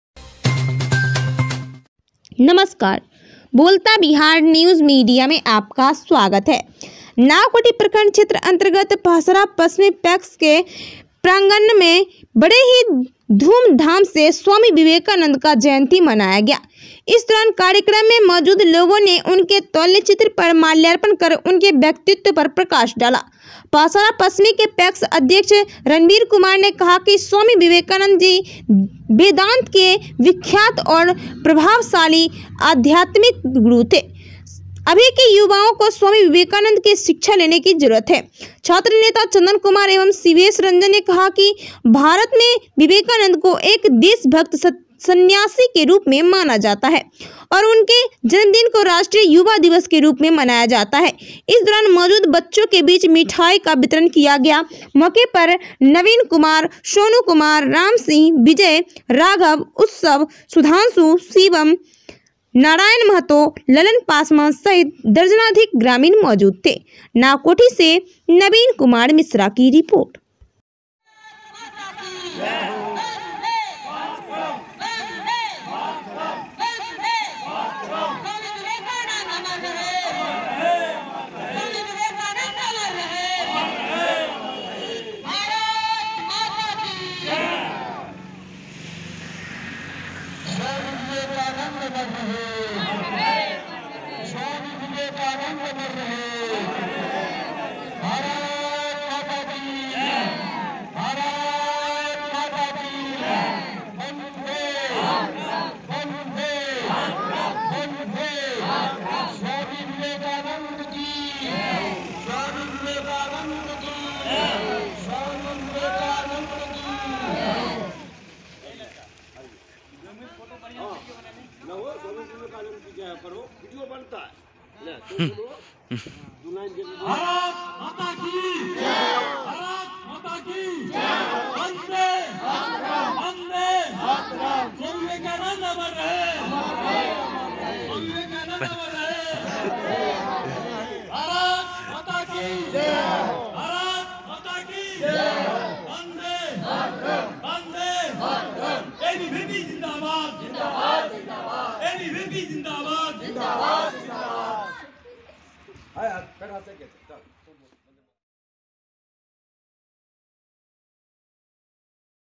पहसारा पश्चिमी पैक्स में मनाया गया स्वामी विवेकानंद जयंती
नावकोठी/बेगूसराय/संवाददाता
प्रखण्ड क्षेत्र अन्तर्गत  पहसारा पश्चिम पैक्स के प्रांगण में बड़े ही धूमधाम से स्वामी विवेकानंद का जयंती मनाया गया।इस दौरान कार्यक्रम में मौजूद लोगों ने उनके तैल चित्र पर माल्यार्पण कर उनके व्यक्तित्व पर प्रकाश डाला।पहसारा पश्चिमी के पैक्स अध्यक्ष रणवीर कुमार ने कहा कि स्वामी विवेकानन्द जी वेदान्त के विख्यात और प्रभावशाली आध्यात्मिक गुरु थे।अभी के युवाओं को स्वामी विवेकानंद से शिक्षा लेने की जरूरत है।छात्र नेता चंदन कुमार एवं सिवेश रंजन ने कहा कि भारत में विवेकानन्द को एक देशभक्त सन्यासी के रूप में माना जाता है और उनके जन्मदिन को राष्ट्रीय युवा दिवस के रूप में मनाया जाता है।इस दौरान मौजूद बच्चों के बीच मिठाई का वितरण किया गया।मौके पर नवीन कुमार,सोनू कुमार, राम सिंह, विजय, राघव, उत्सव, सुधांशु, शिवम, नारायण महतो,ललन पासवान सहित दर्जनाधिक ग्रामीण मौजूद थे।